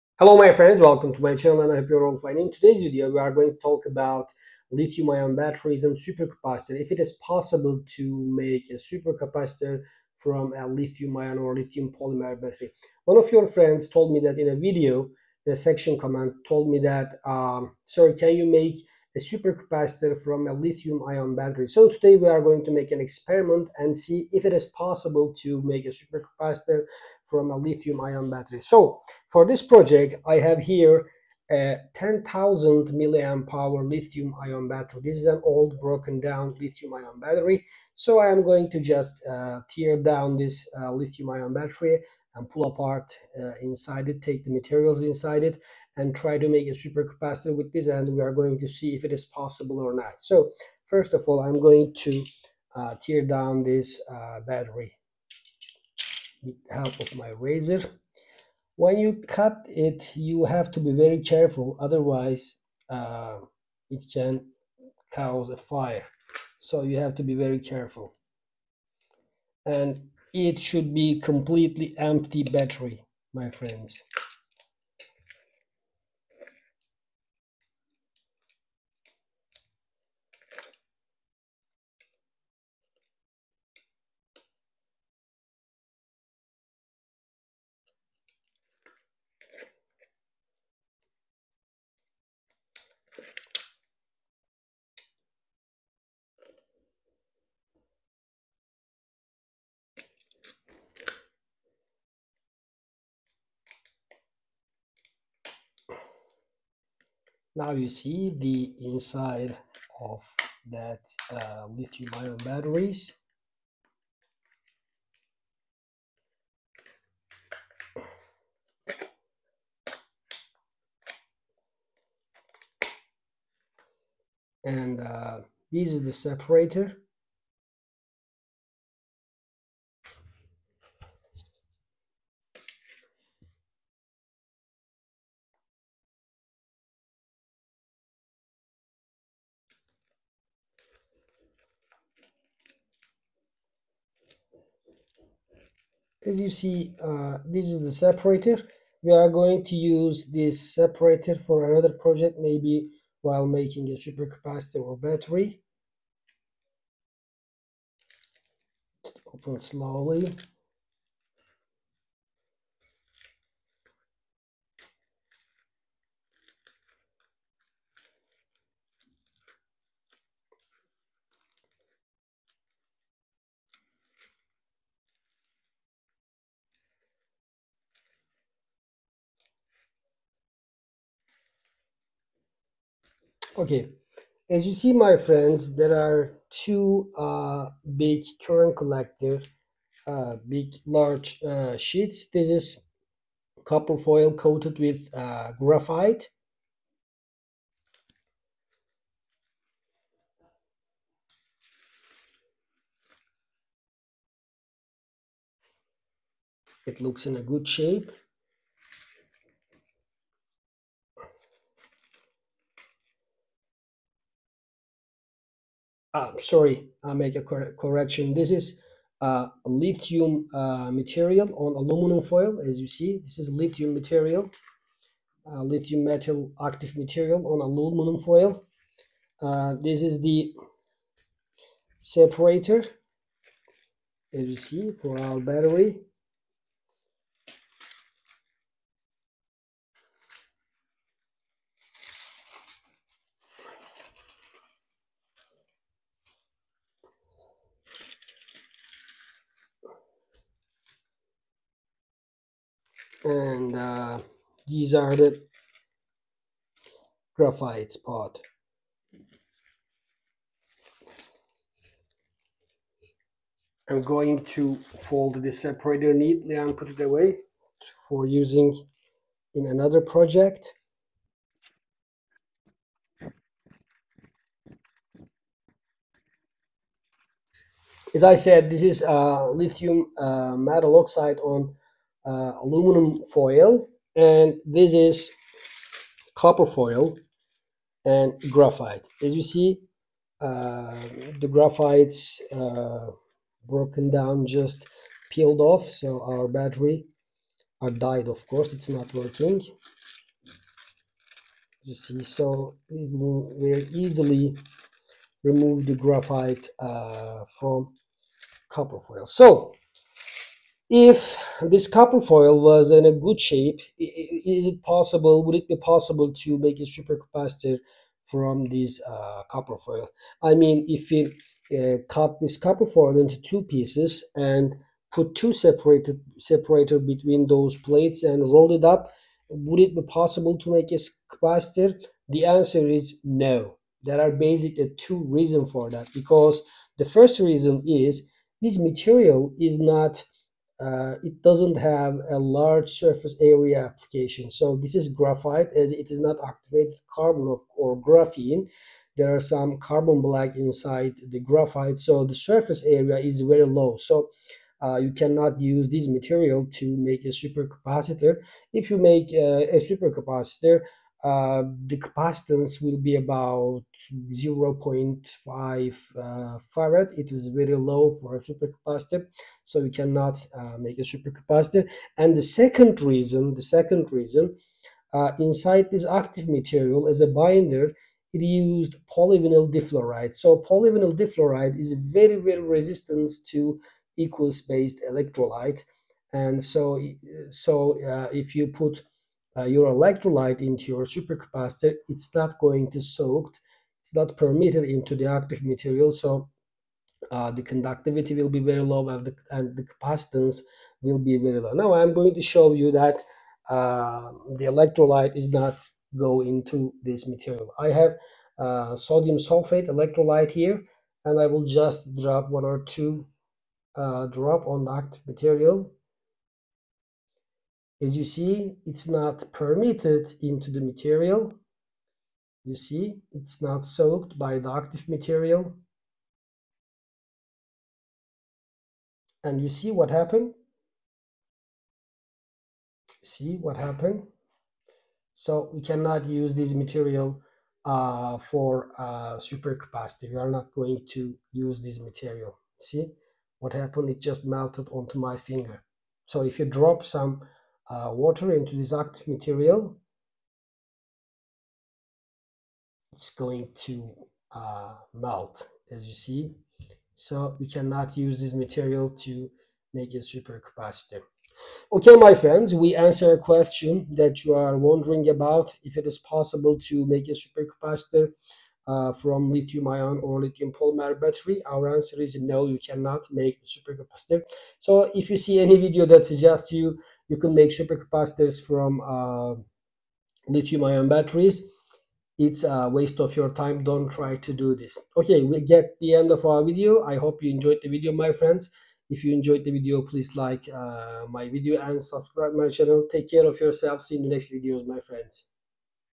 Is it Possible to Make a Supercapacitor from a Lithium Ion Battery?
In this video video we will try to make a supercapacitor from a lithium ion, lithium polymer battery and try to figure out if it is possible or not. First of all we will disassemble, tear down, cut a lithium battery, look inside it and then we will make some experiments to see is it possible to make supercapacitor. I will tell you the full explanation.